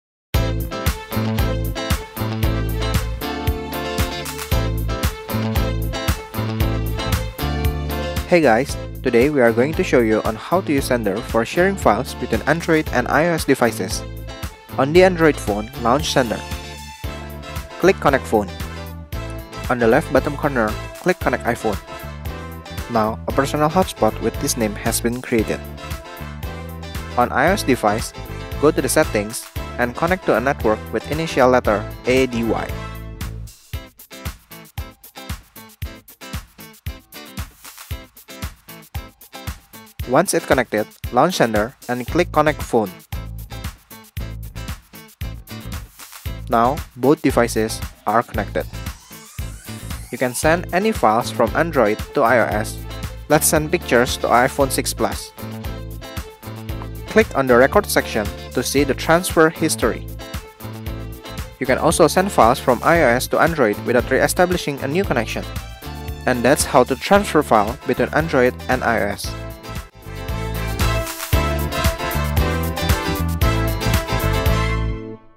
Xender - How to Transfer Files Between Android and iOS
While most file transfer apps require the same platform to function, Xender breaks this limitation. Xender allows you to transfer any file of any size from Android to iOS and vice versa. No cable, computer or Internet connection needed to perform this feature. Xender is an absolute help for all smartphone users.